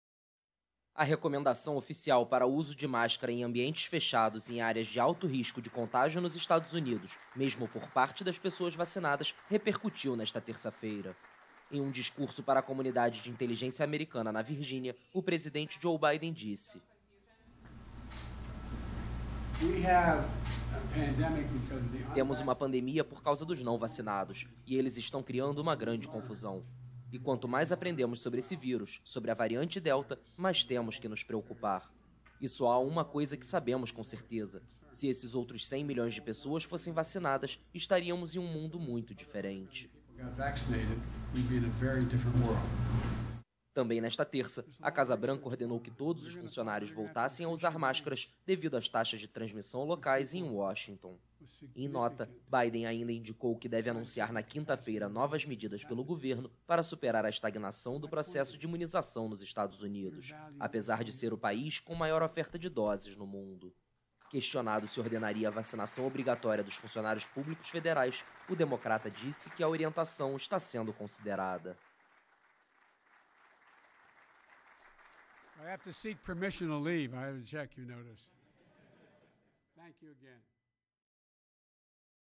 EUA voltam a recomendar máscaras em lugares fechados para pessoas vacinadas
O avanço da variante Delta nos Estados Unidos preocupa autoridades sanitárias, que lançaram novas recomendações nesta terça-feira. O presidente Joe Biden disse que o país deve 'melhorar' as taxas de vacinação e que 'considera' impor a imunização obrigatória para os dois milhões de funcionários federais.